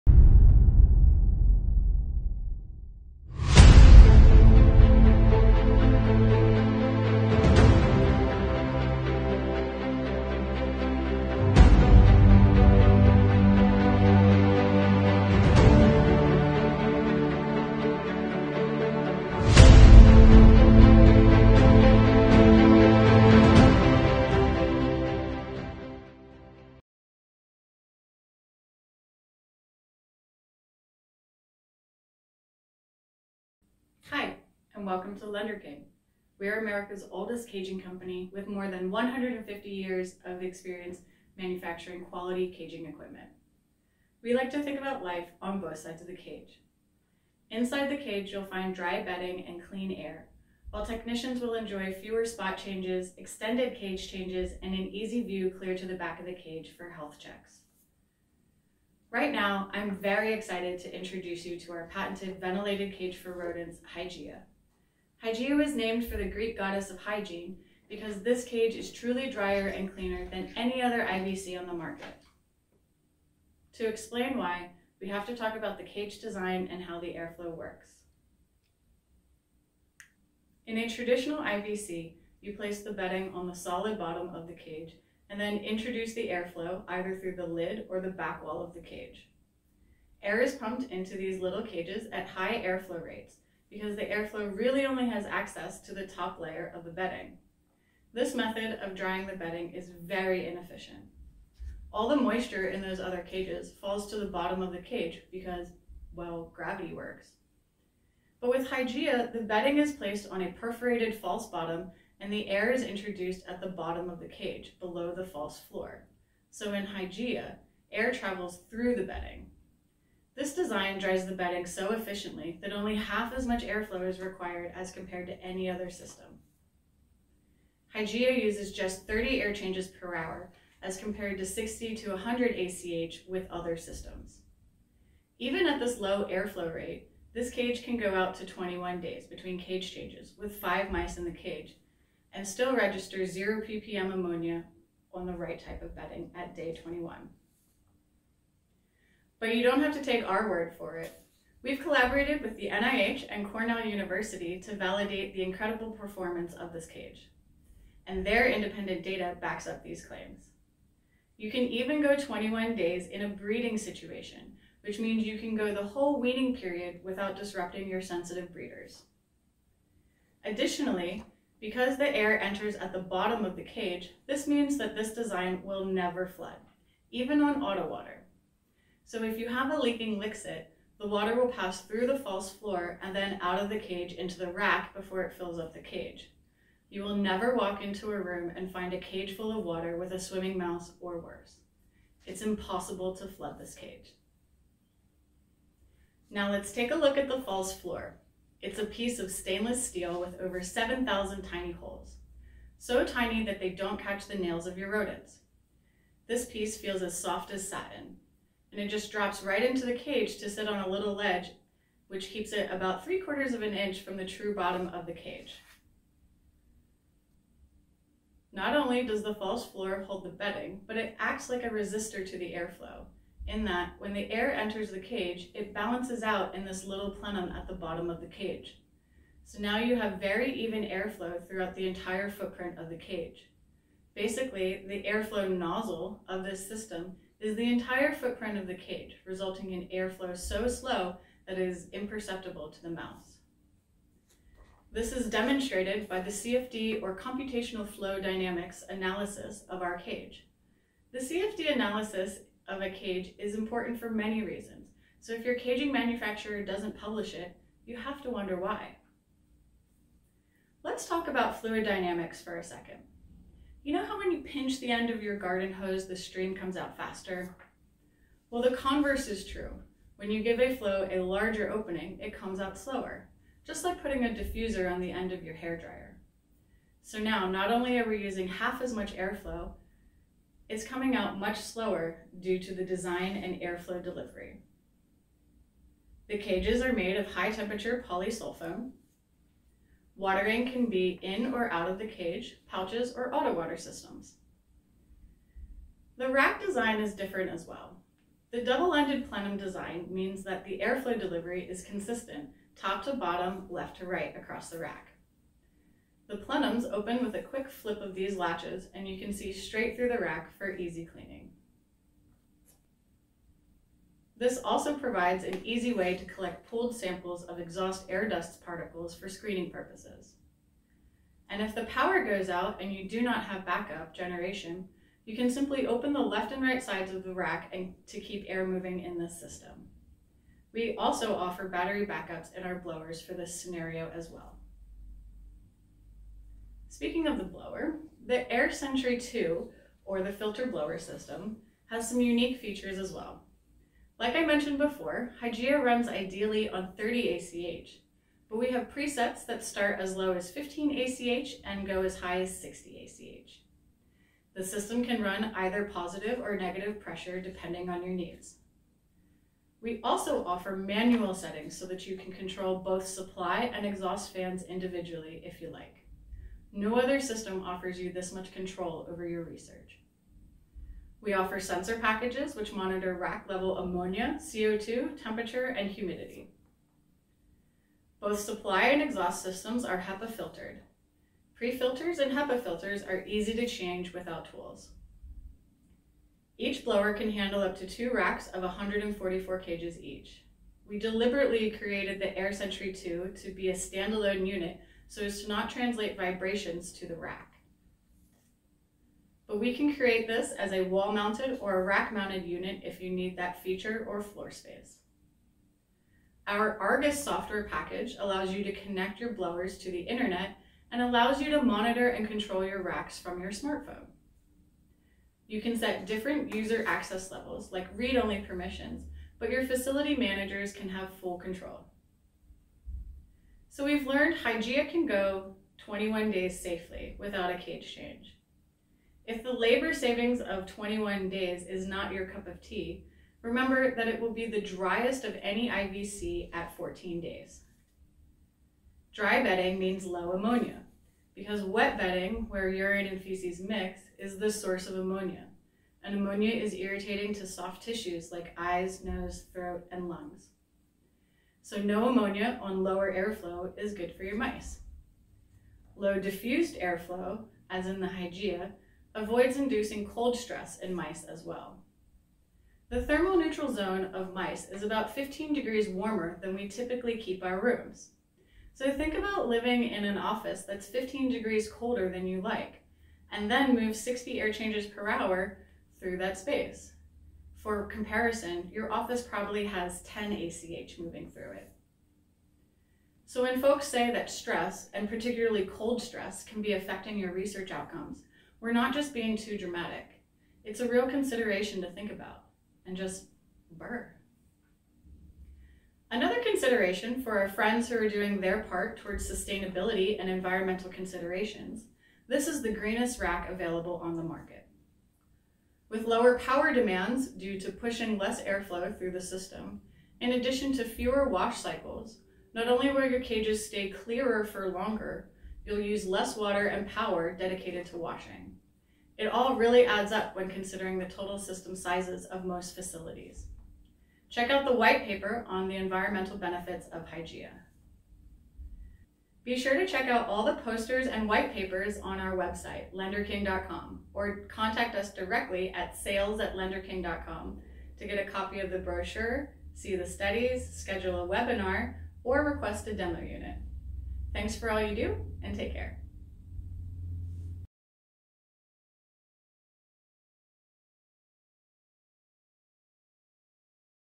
Hygieia, Lenderking's Revolutionary Individually Ventilated Cage
Hygieia is the industry's first design revolution in individually ventilated cages for rodent since the concept was invented. The unique "Flow Thru" bedding concept keeps the bedding drier and can keep a cage with 5 mice ammonia free for 21 days.